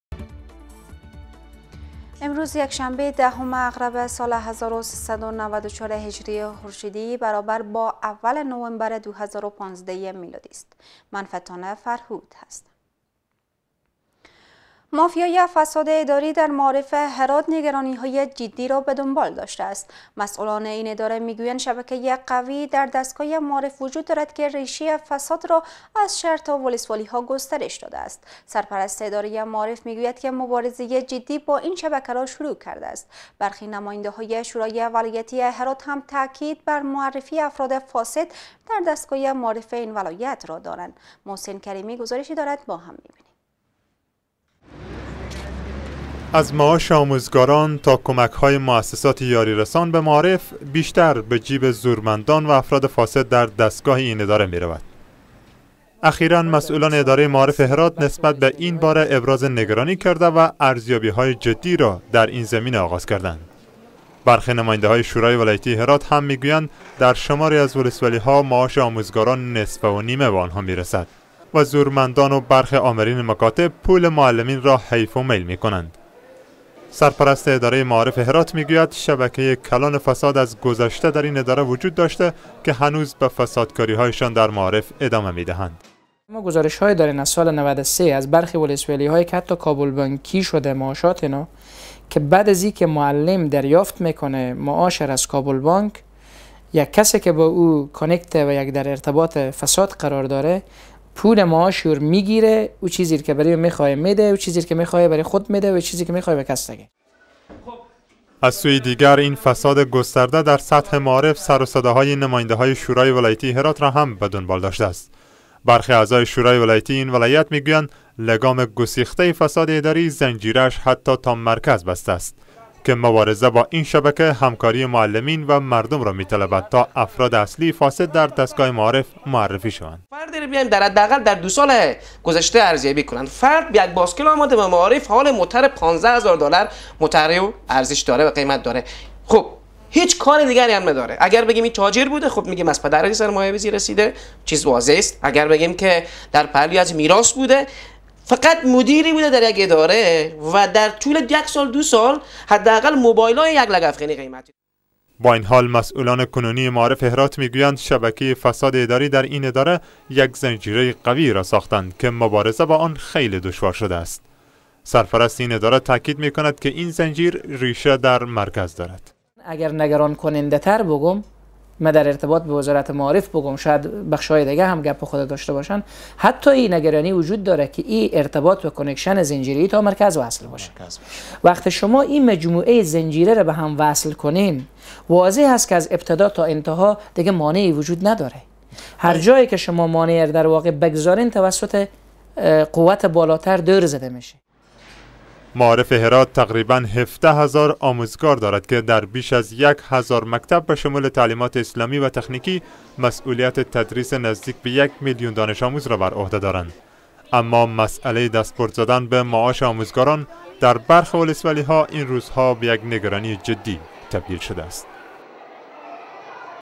نگرانی از مافیای فساد اداری در دستگاه معارف هرات
مافیای فساد اداری در معارف هرات نگرانی های جدی را به دنبال داشته است. مسوولان این اداره میگویند شبکه قوی در دستگاه معارف وجود دارد که ریشه فساد را از شهر تا ولسوالی ها گسترش داده است. سرپرست اداره معارف میگوید که مبارزه جدی با این شبکه را شروع کرده است. برخی نماینده های شورای ولایتی هرات هم تاکید بر معرفی افراد فاسد در دستگاه معارف این ولایت دارند.